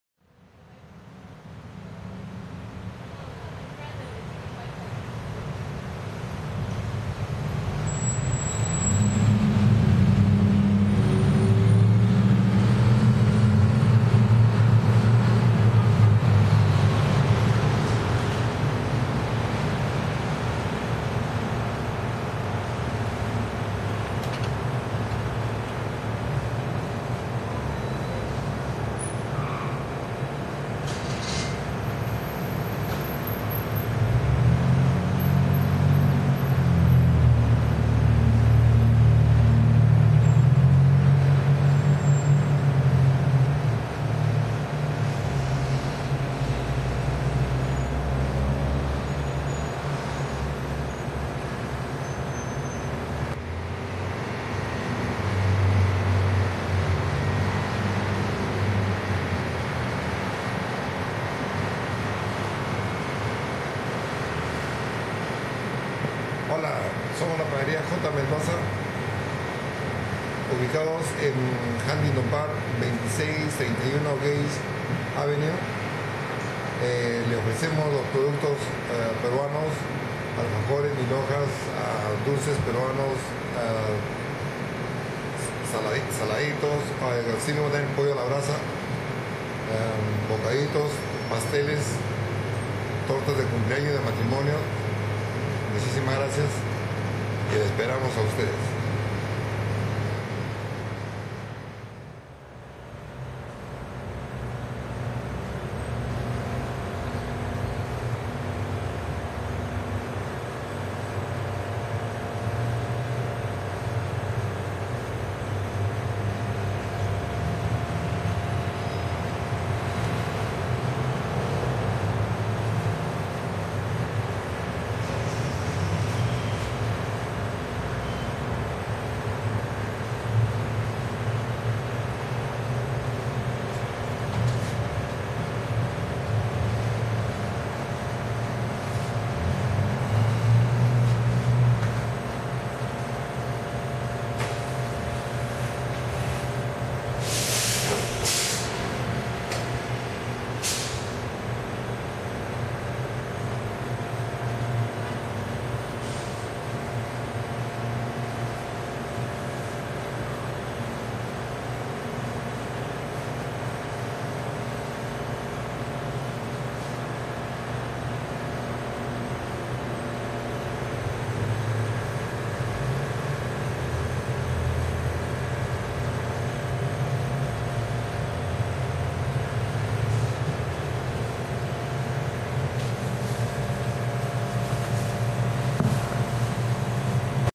Panadería Peruana ( Pollo a la Brasa ) en HUNTINGTON PARK, CA
Panadería Peruana J. Mendoza los invita a saborear la riquísima comida peruana y pollo a la brasa en Huntington Park, CA. Tambien tenemos panes, pasteles para fiestas, quinceañeras, bodas, etc. Disfrute los mas ricos postres como alfajores, crema volteada, gelatina, panetón y mucho más.Estamos ubicados en 2631 E. Gage Ave.